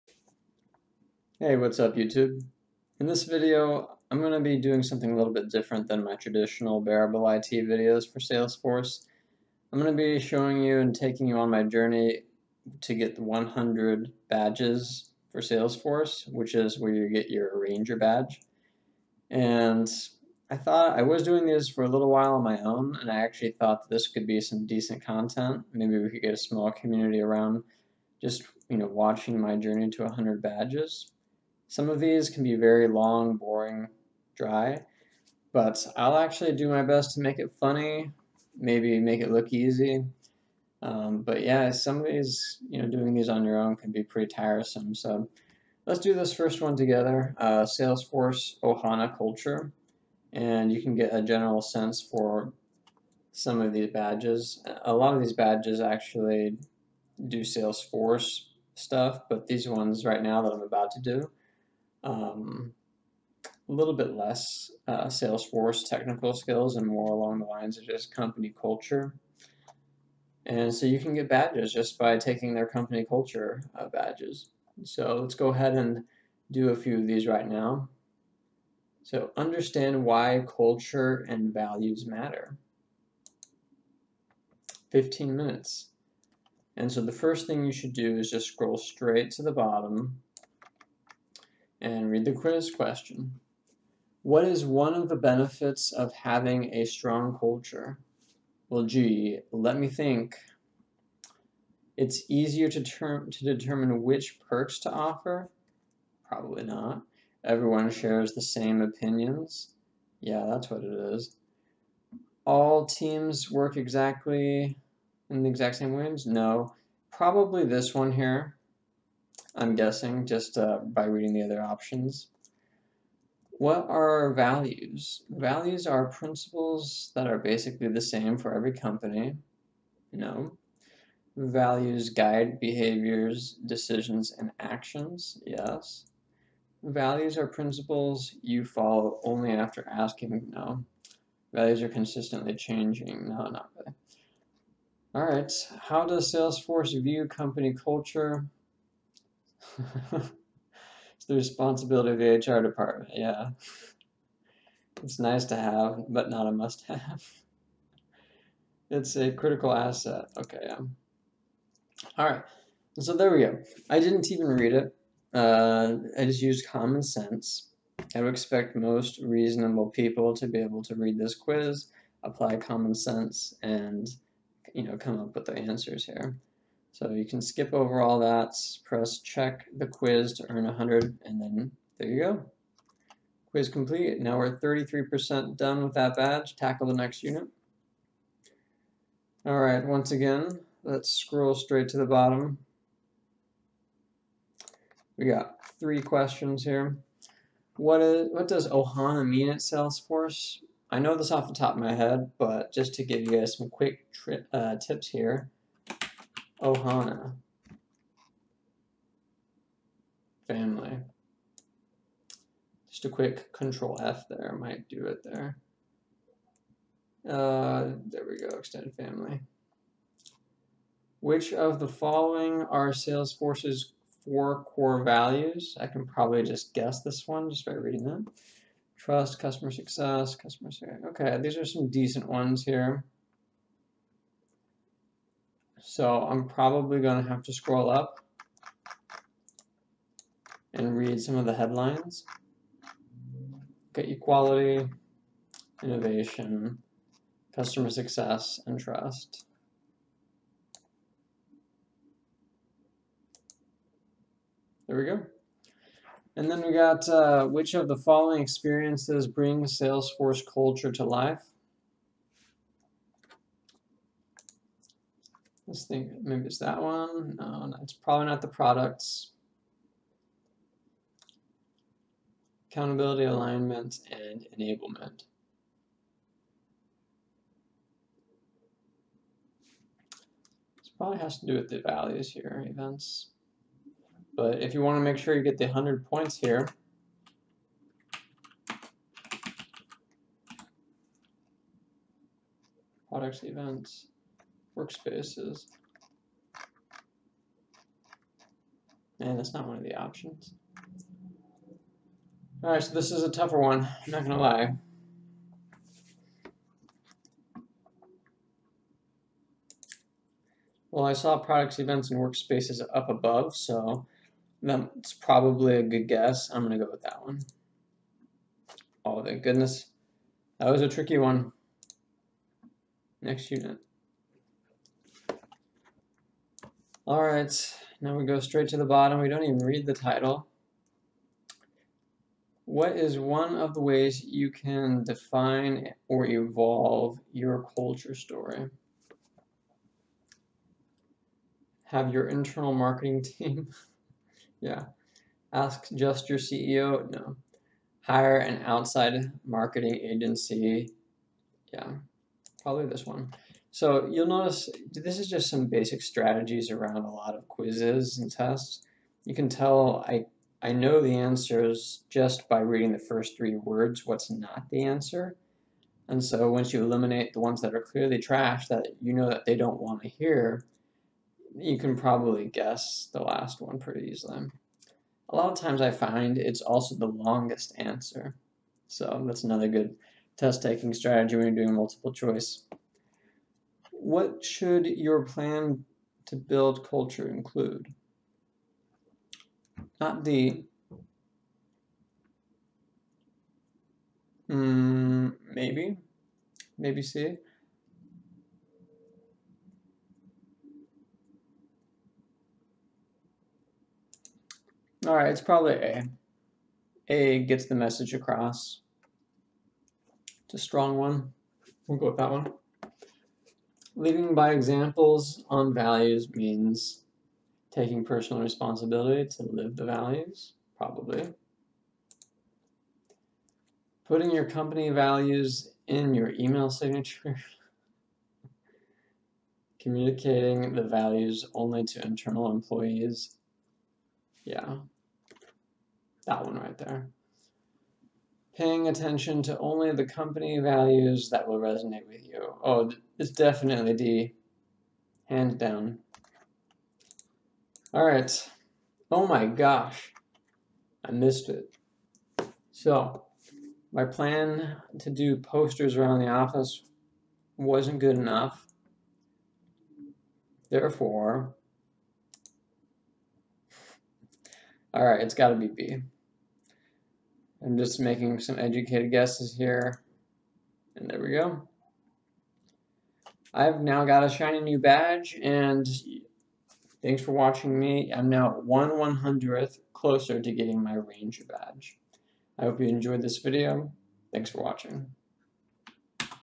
Salesforce Ohana Culture Trailhead (Journey to Ranger)
In this video I will show you how to get your Salesforce Ohana Culture Badge